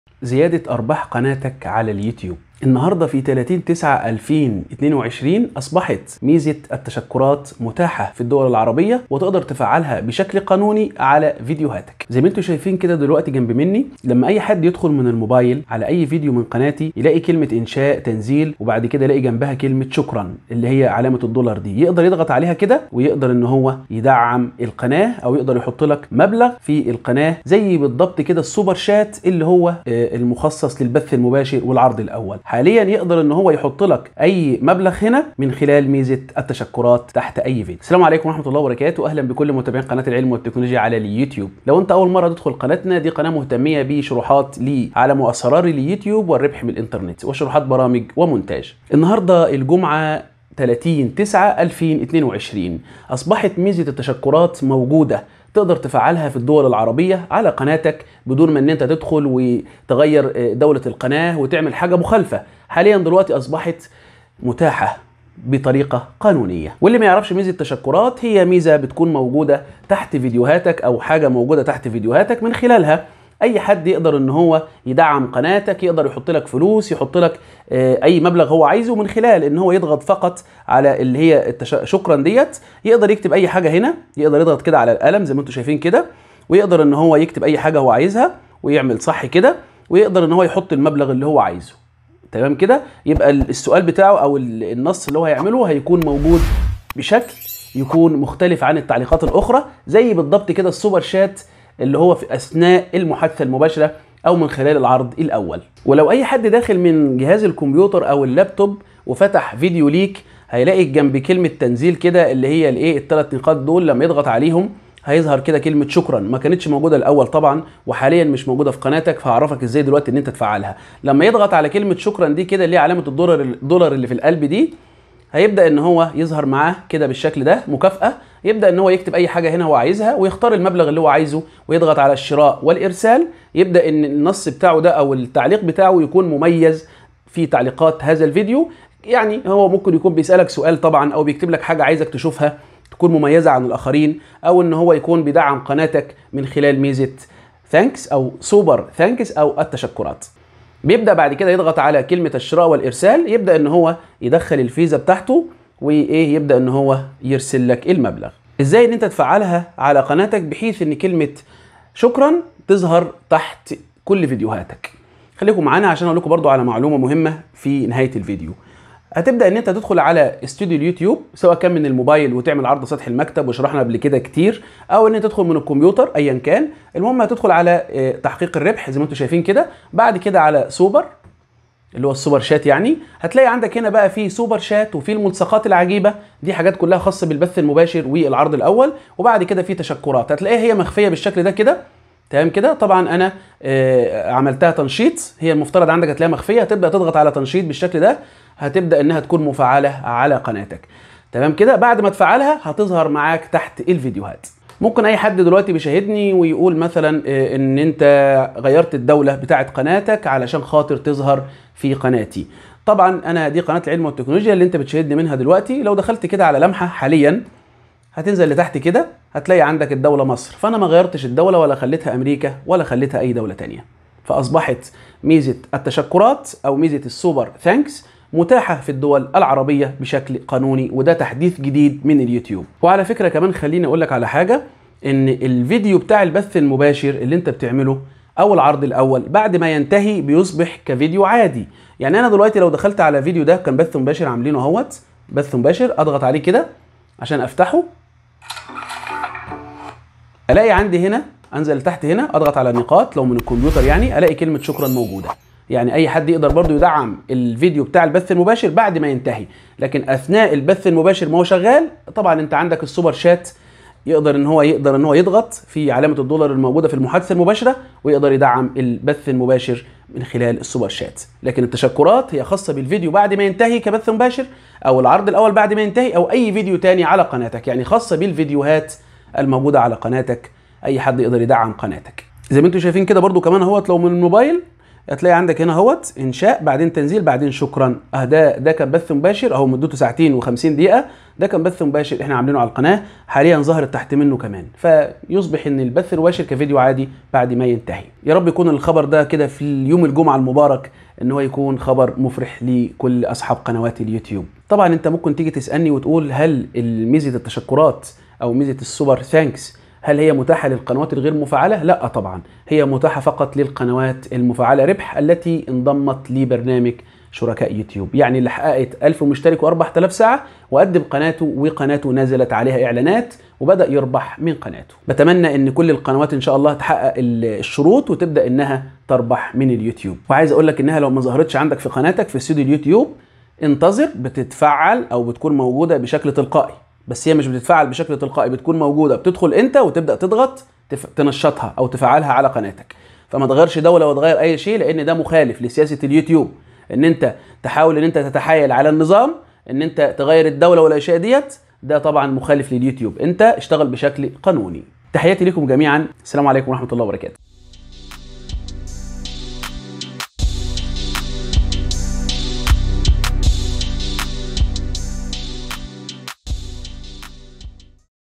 تحديث جديد🔥تفعيل ميزة تشكراتSuper Thanks🤑زيادة أرباح قناة اليوتيوب2023| فعلها الان
أصبحت الان ميزة تشكراتSuper Thanks متاحة في قنوات اليوتيوب ، نوضح تفعيل ميزة Super Thanks شكرا ،من خلال الميزة "شكرًا" (المعروفة سابقًا باسم "تصفيق الجمهور")، يمكن لمنشئ المحتوى الحصول على مصدر جديد للأرباح والتواصل مع المشاهدين الذين يريدون التعبير عن تقديرهم لفيديوهاته ،نوضح تفعيل ميزة تشكرات لزيادة أرباح قناة اليوتيوب.يمكن للمشاهد من خلال ميزة تشكراتSuper Thanks شراء صورة متحركة ممتعة تُسمّى "شكرًا" على صفحة فيديو معيّن. يتم إرسال الصورة المتحركة مرة واحدة، وهي تظهر على سطح الفيديو أمام المستخدم الذي اشتراها فقط.وسيتمكن المشتري أيضًا من نشر تعليق مميّز وملوّن وقابل للتخصيص في قسم التعليقات الخاص بالفيديو.
مقالة بها كل ما يخص ميزة التشكرات
وسائل التواصل
قناتنا الجديدة على التليجرام
قناتنا الثانية على اليوتيوب
Youtube.com/Mohanysoliman2